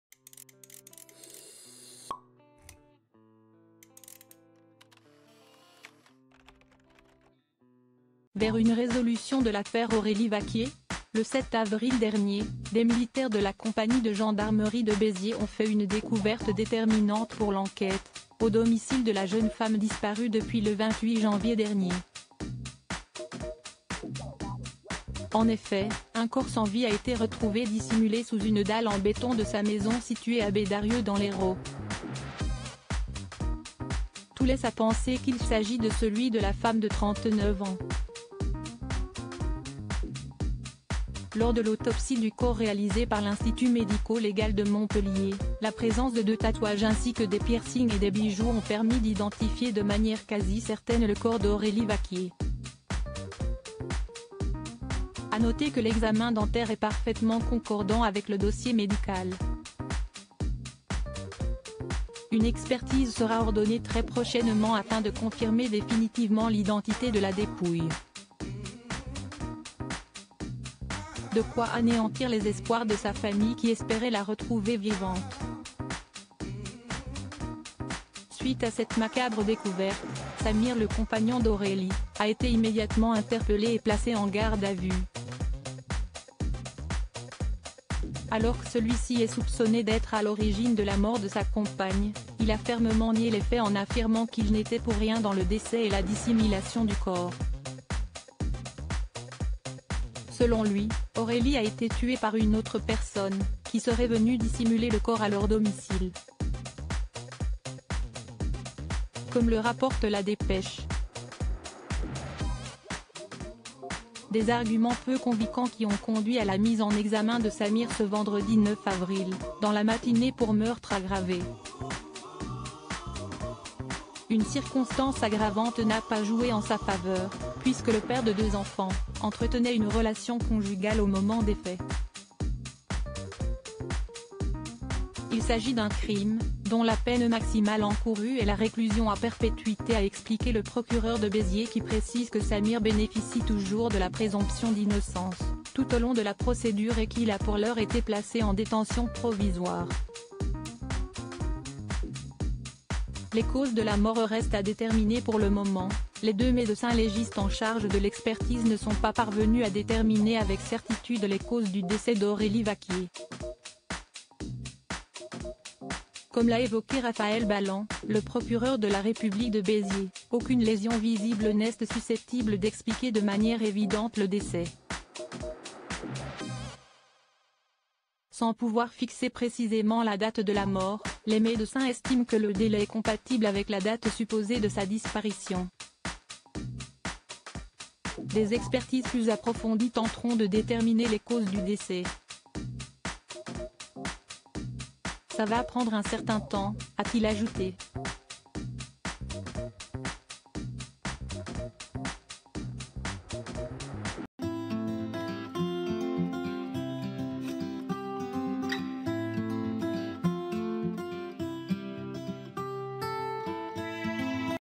Aurélie Vaquier : son compagnon mis en examen pour meurtre aggravé, le procureur fait des révélation
Aurélie Vaquier : son compagnon mis en examen pour meurtre aggravé, le procureur fait des révélations